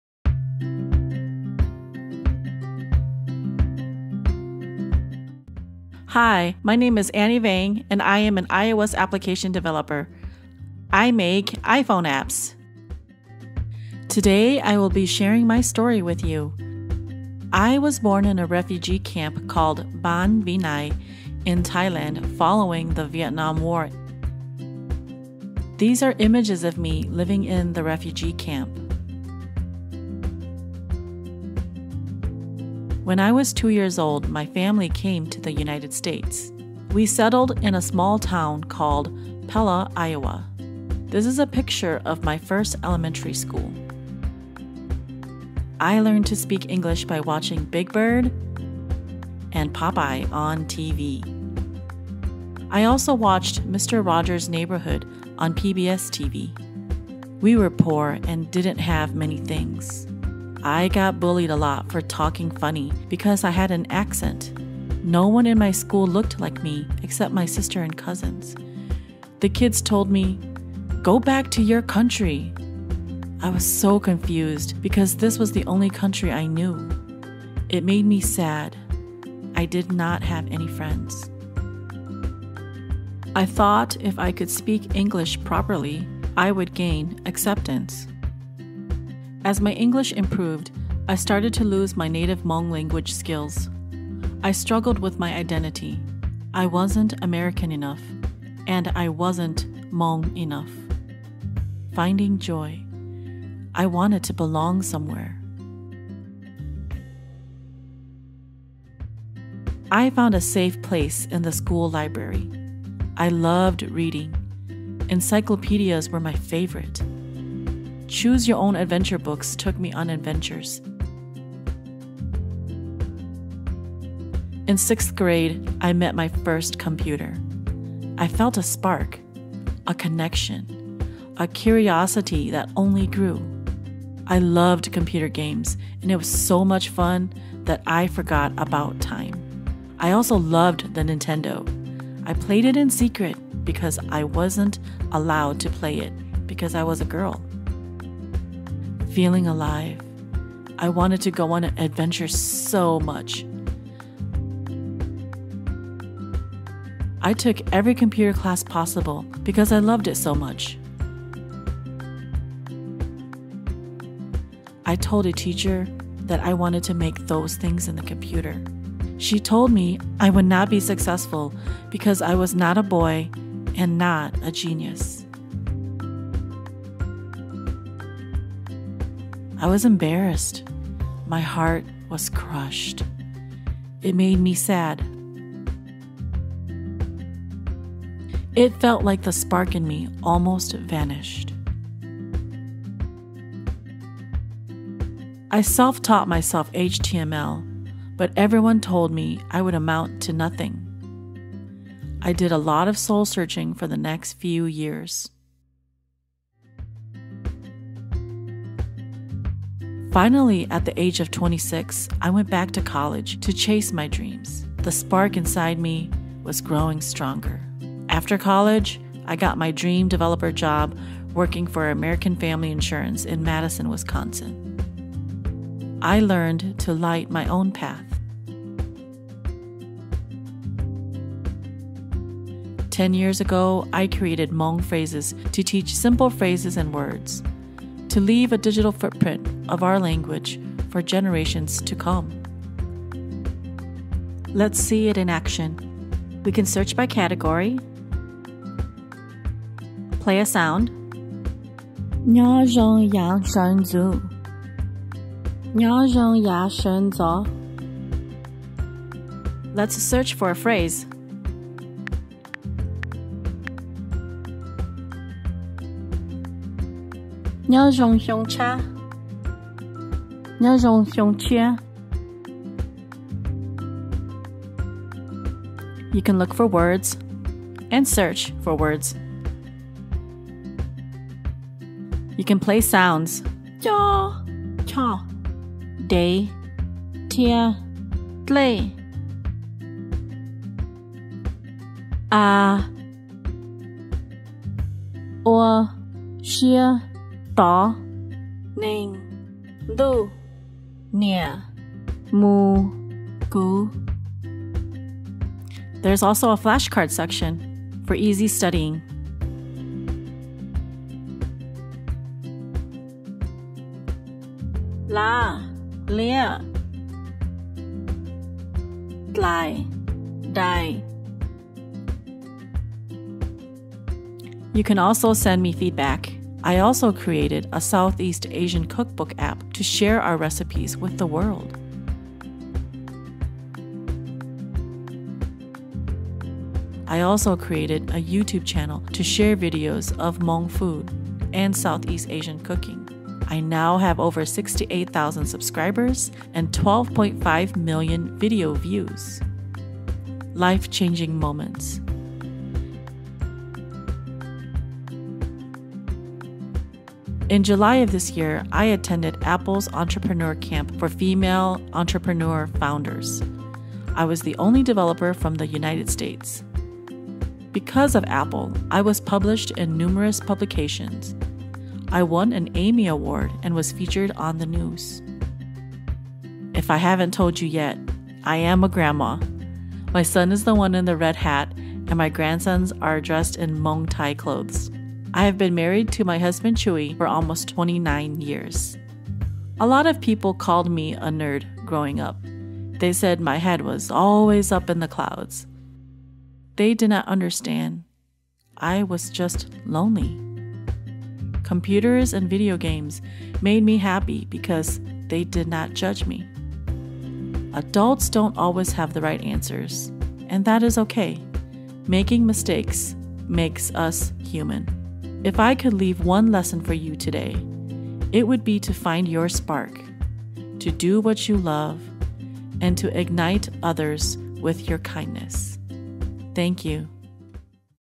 Finding your passion. Ignite others with your kindness.
This is a short video about my journey into STEM.

Audience: students, elementary, middle, high school